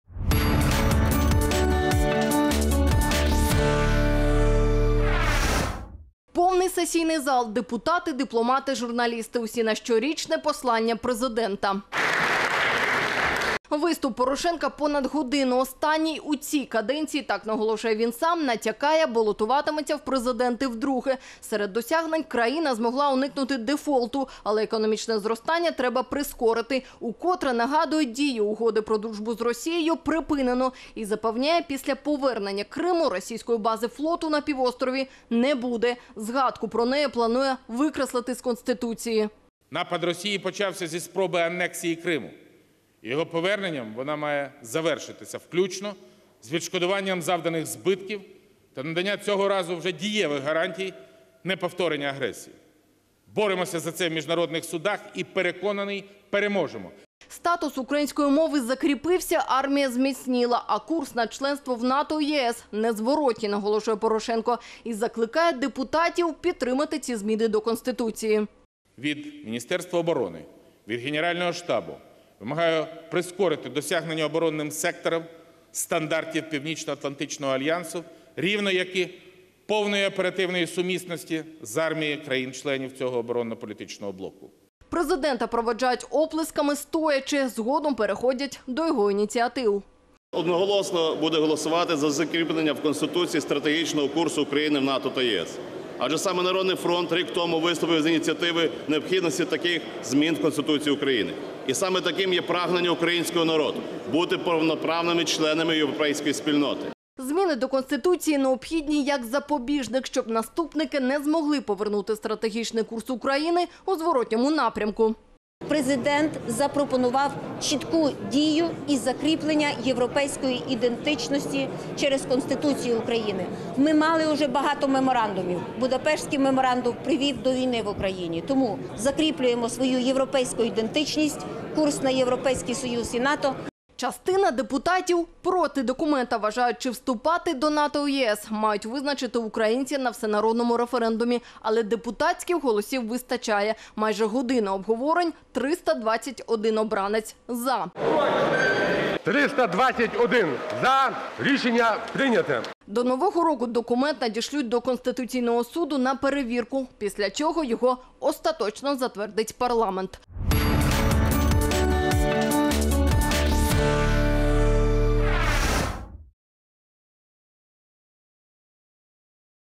Петро Порошенко про досягнення України
Виступ Порошенка - понад годину. Останній у цій каденції – так наголошує він сам. Натякає - балотуватиметься в президенти вдруге. Серед досягнень – країна змогла уникнути дефолту, але економічне зростання треба прискорити. Укотре нагадує: дію угоди про дружбу з Росією - припинено.